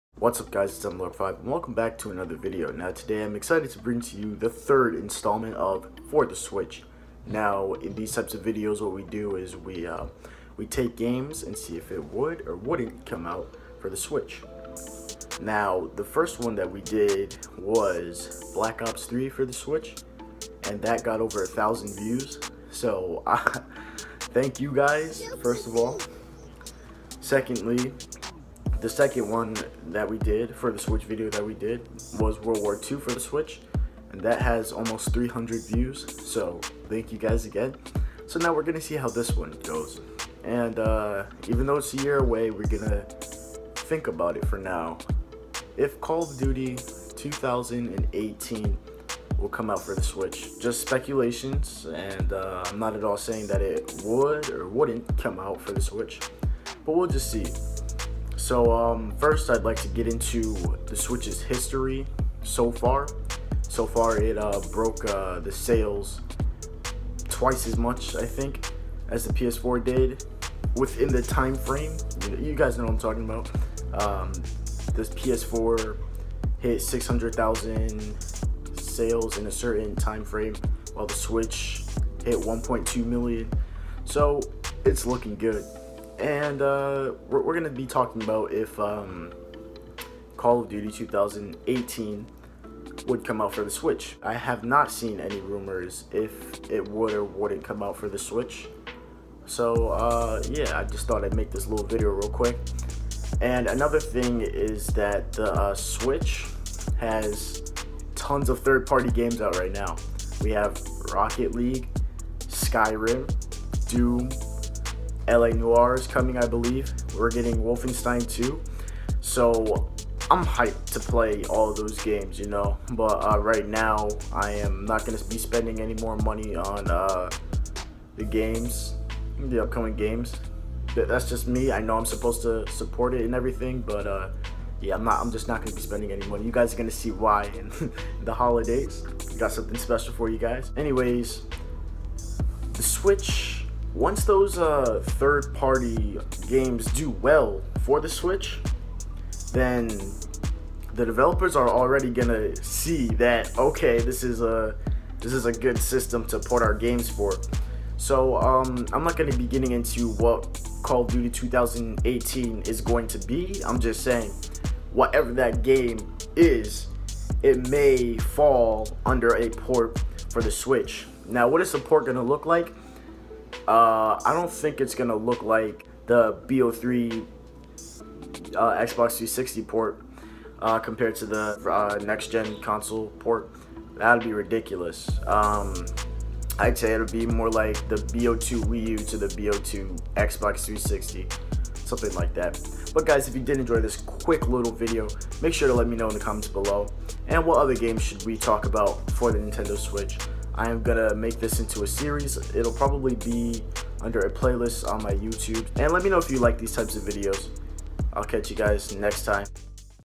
CoD 2018 for Nintendo Switch?
Please read my about page for more information!

This video was not aimed at trying to figure out what CoD 2018 is but whatever the game is, if it would be for the Nintendo Switch!